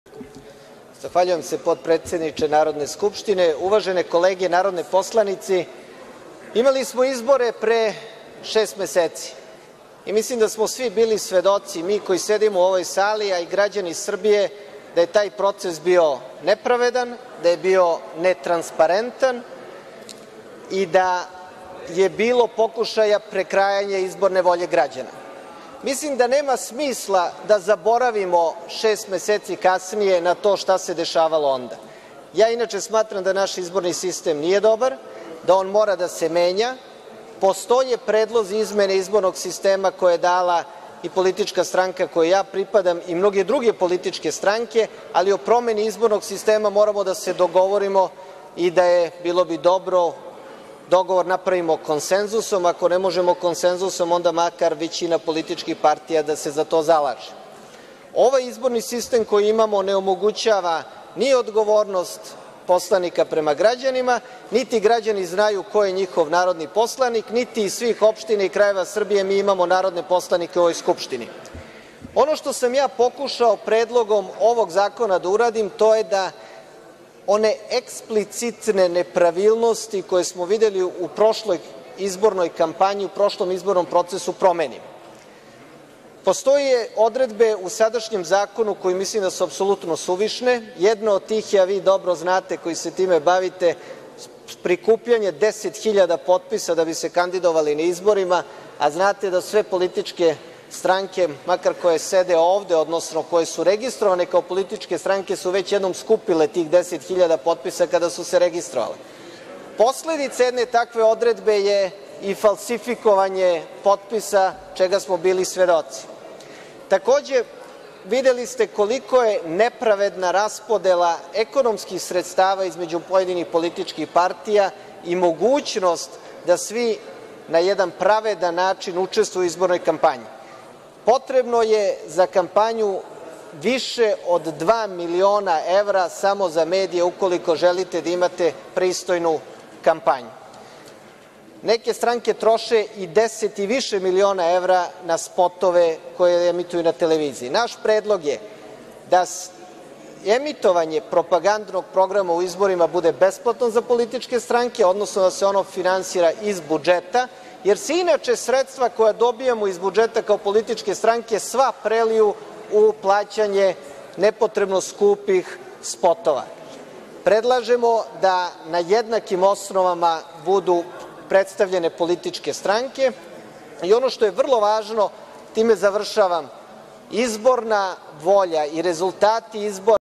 Nenad Konstantinović SDS - Neophodne promene izbornog sistema (Oktobar, 2016.)
Narodni poslanik Socijaldemokratske stranke Nenad Konstantinović.

Beograd,
Oktobar, 2016.